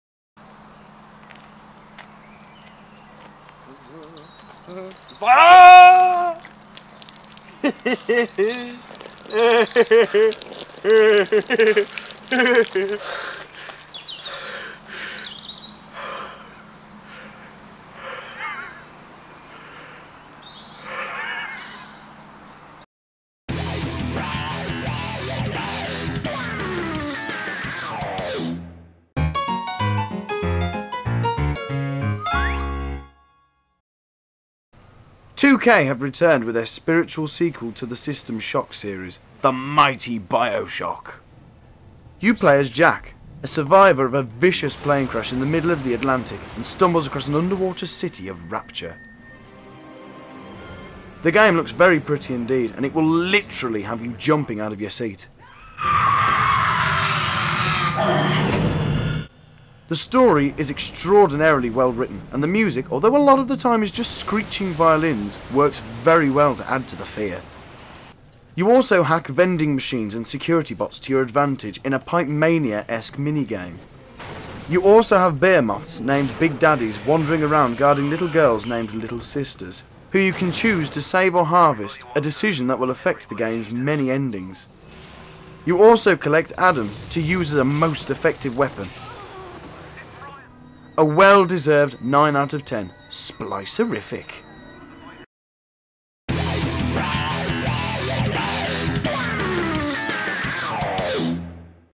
Bioshock (PS3) review - 60 Second Gamer - Episode 13
Part 2 of 3 in today's updates. This review covers Bioshock from 2K Games/Irrational Games. Enter Rapture for an adventure under the sea. No Little Mermaids or talking lobsters to be seen.